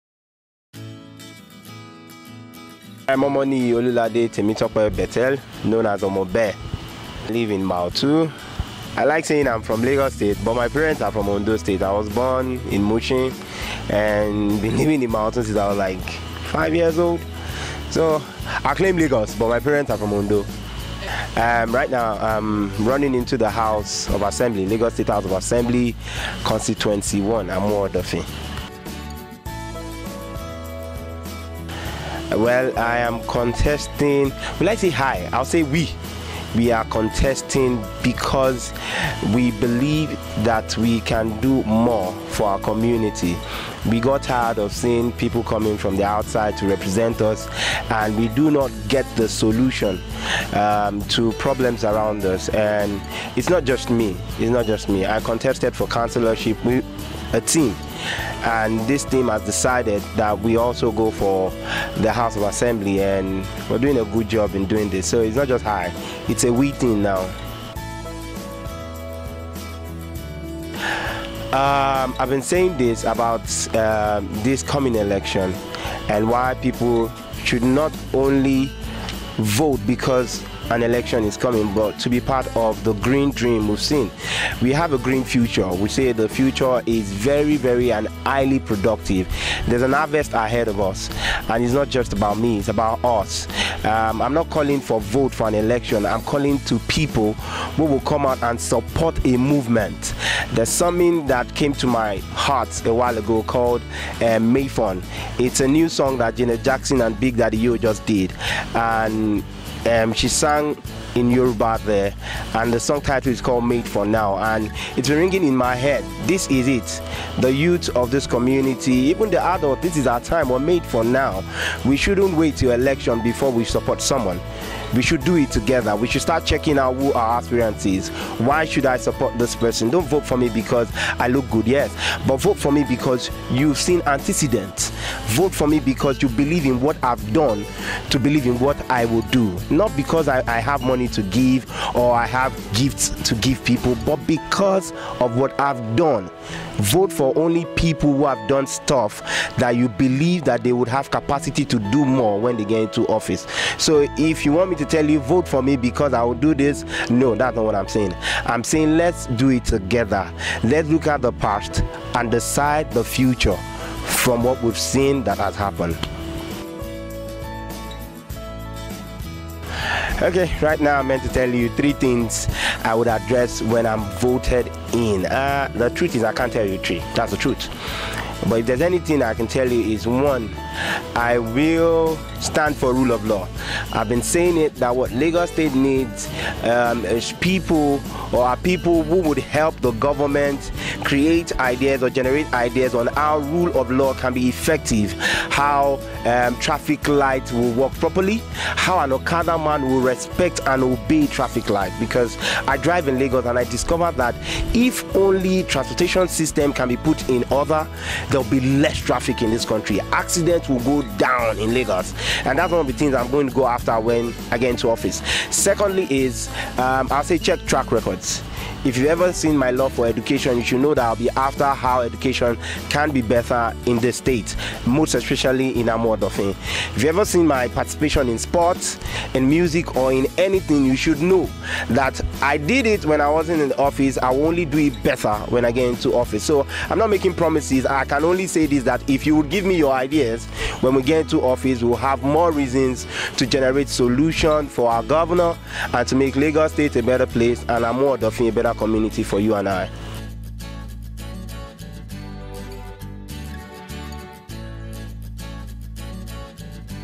Amuwodecides 2019 Know Your Aspirant Episode 2 : OMONIYI OLU BETHEL
Festaconline TV, FOTV  Brings to you the second series of  Know Your Aspirant Video Series where we get to meet and know the individuals aspiring for governing, Legislative positions in our community, Amuwo-Odofin LGA, Lagos State.

In anticipation of the Scheduled 2019 Nigeria General Elections to be held in February 2019.

 Meet House of Assembly Aspirant Mr Olulade Bethel for Amuwo Odofin Constituency 1.

Your Vote Counts